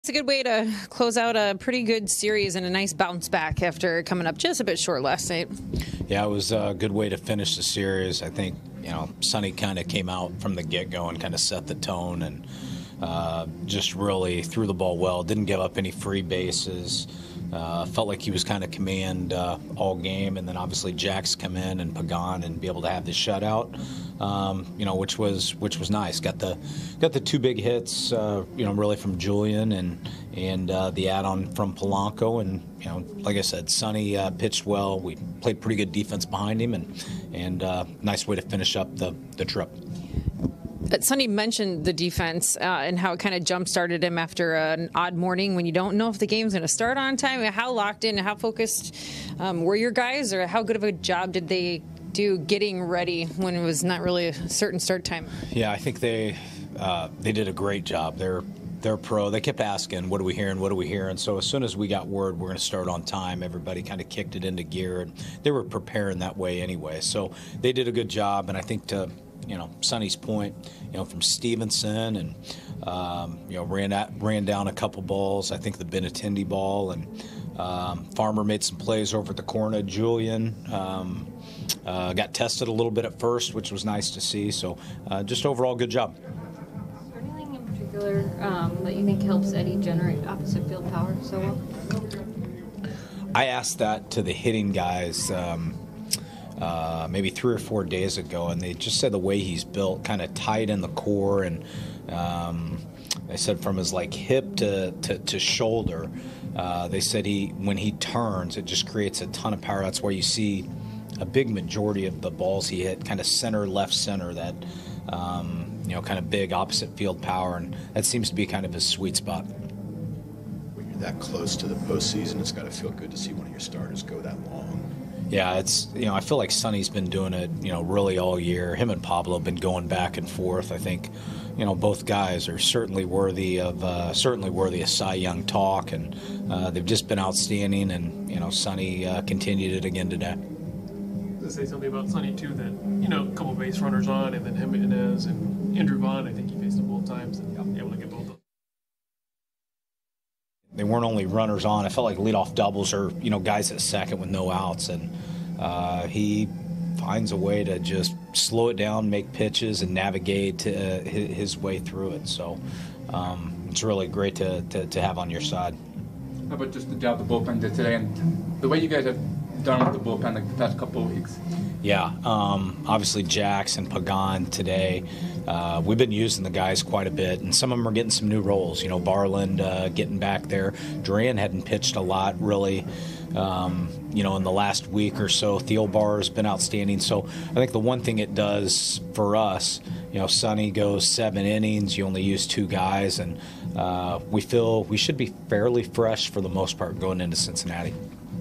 Twins acting manager Jayce Tingler after series win in Chicago
Minnesota Twins acting manager Jayce Tingler after the series win in Chicago.